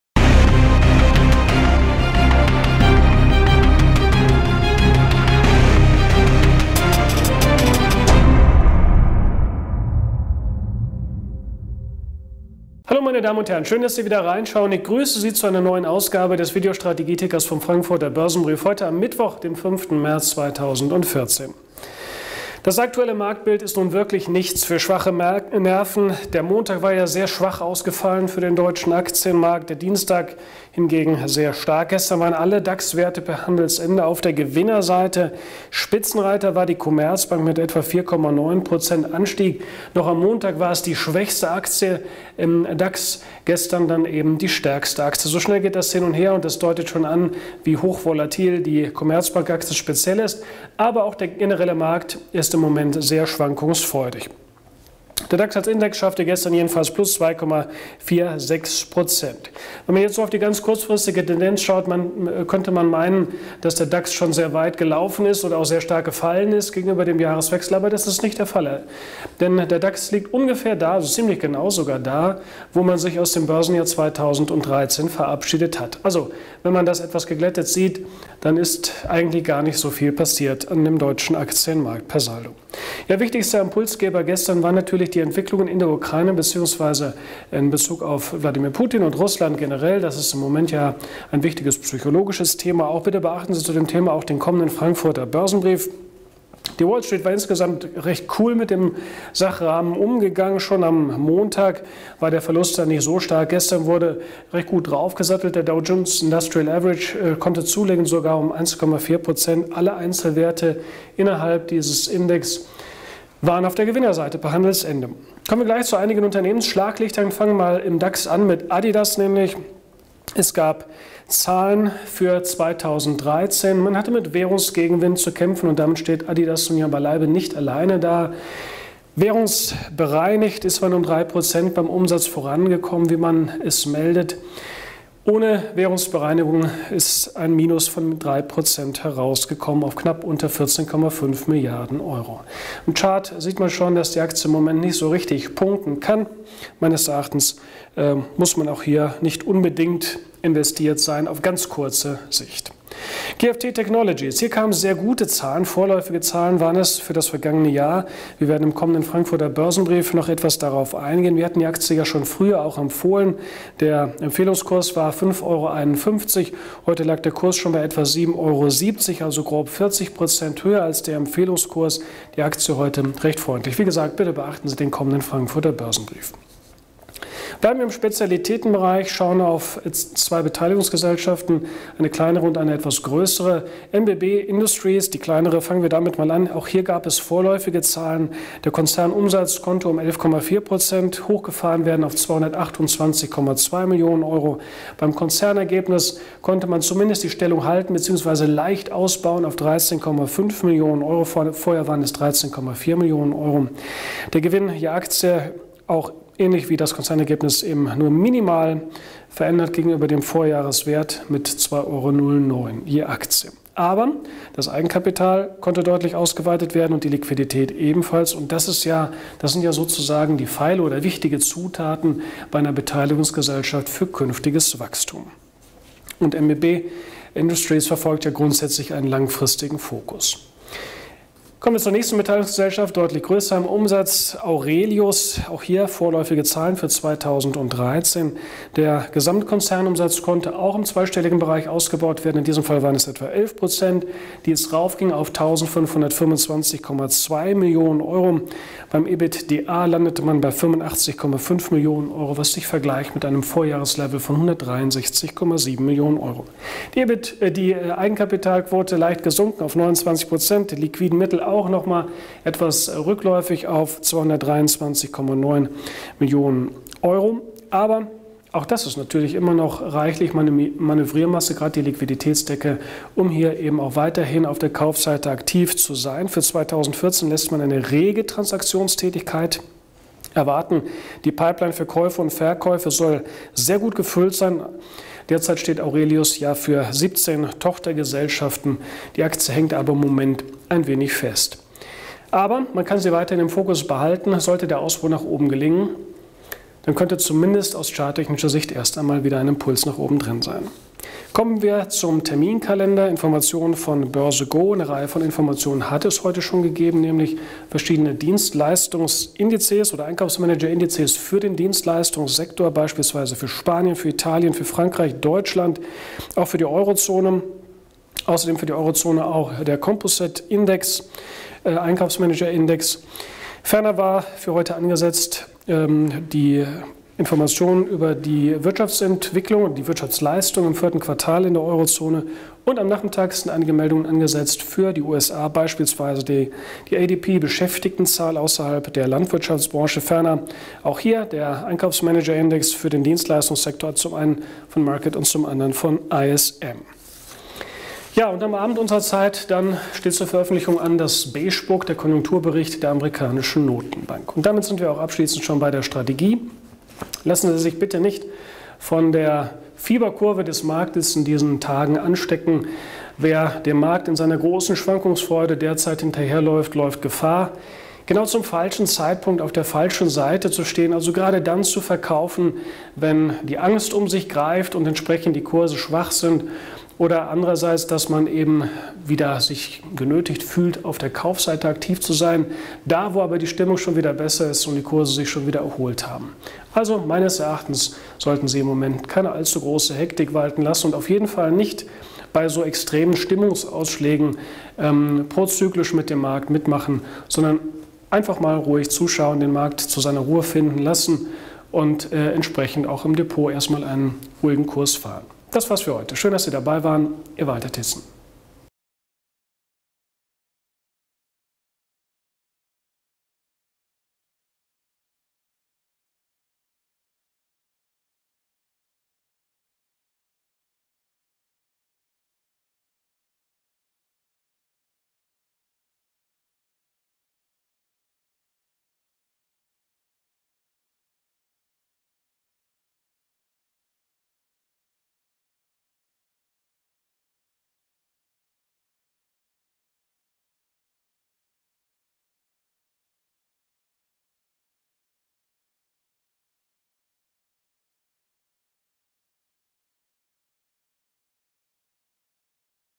05.03.2014 - Videoticker Frankfurter Börsenbrief
Bleiben Sie ruhig am Spielfeldrand!
Aktien: Adidas, GFT Technologies, MBB Industries & Aurelius